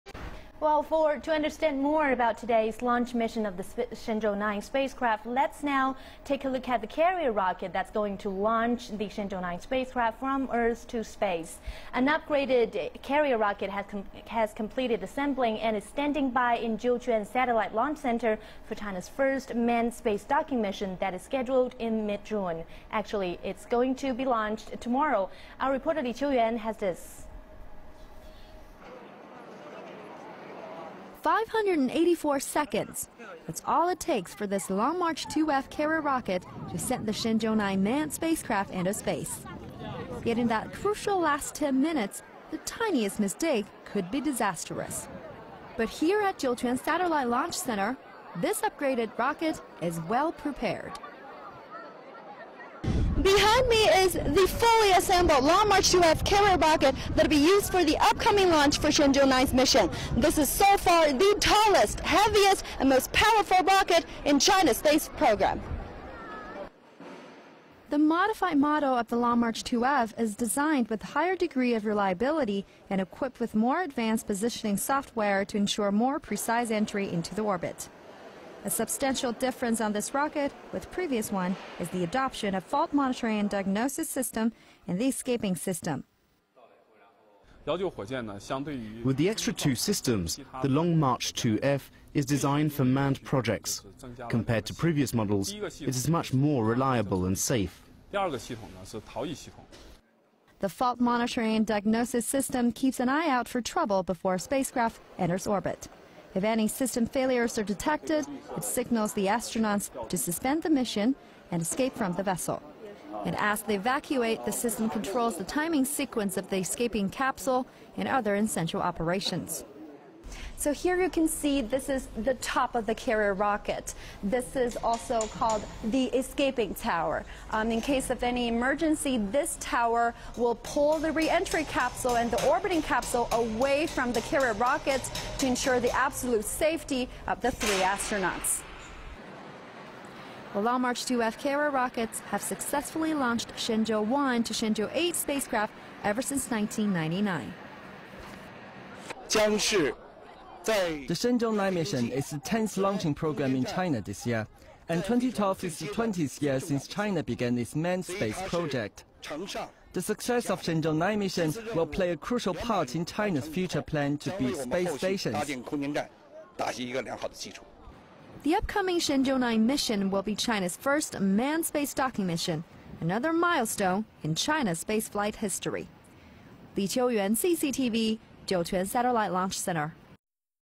China's upgraded Long March 2F carrier rocket system
The modified model of the Long March-2F is designed with a higher degree of reliability and equipped with more advanced positioning software to ensure a more precise entry into the orbit.

A substantial difference on this rocket with previous one is the adoption of the "fault monitoring and dianosis system" and the "escaping system".

Chief disigner of Longmarch-2F carrier rocket Zhang Xuhui says, "With the two extra systems, the Long-March-2F is designed for manned projects. Compared to previous models, it is much more reliable and safe. "

The fault monitoring and diagnosis system keeps an eye out for trouble before the spacecraft enters orbit. If any system failures are detected, it signals the astronauts to suspend the mission and escape from the vessel.

And as they evacuate, the system controls the timing sequence of the escape capsule and other essential operations.